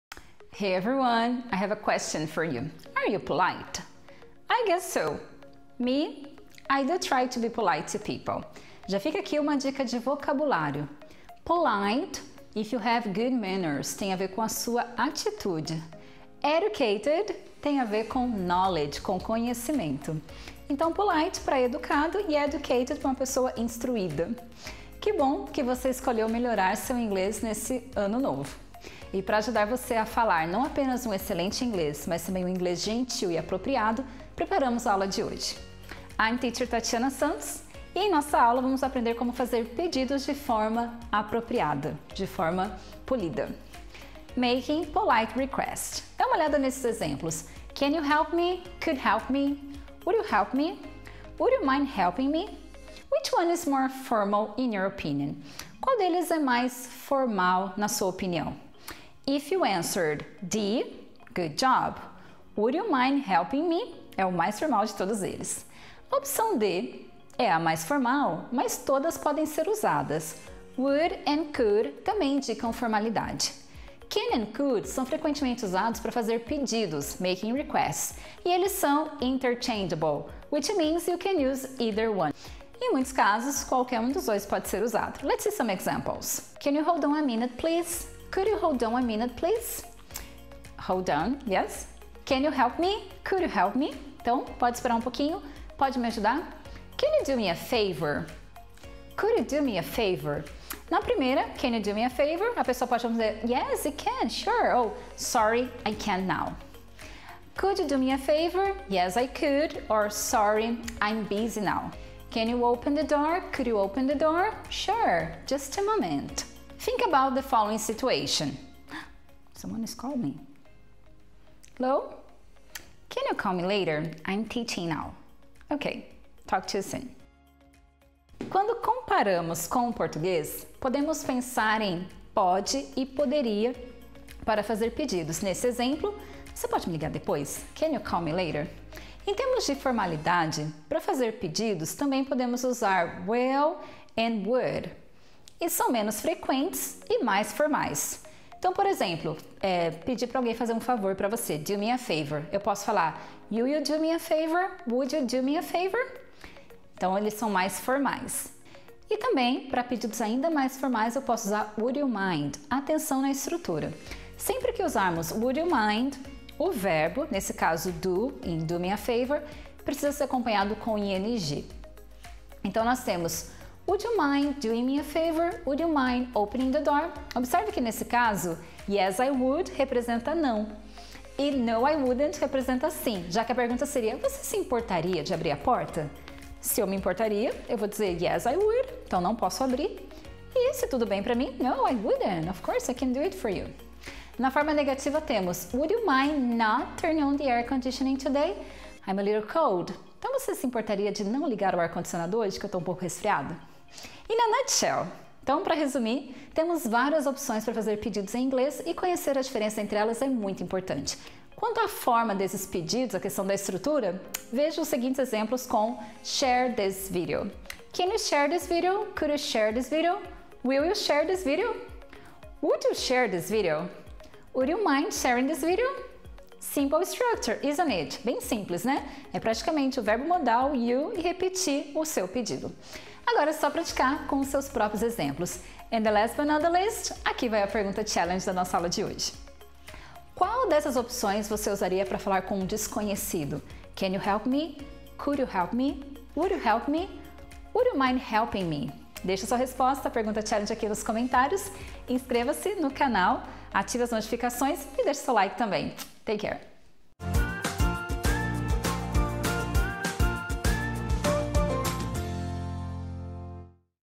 Como fazer pedidos em Inglês? (Modals)
Os modais são super importantes para um Inglês mais fluído, então dá play no vídeo e vamos aprender um pouco mais sobre como fazer pedidos em Inglês!

Qual dessas opções você usaria para falar com um desconhecido?
Could you help me?
Would you help me?
Would you mind helping me?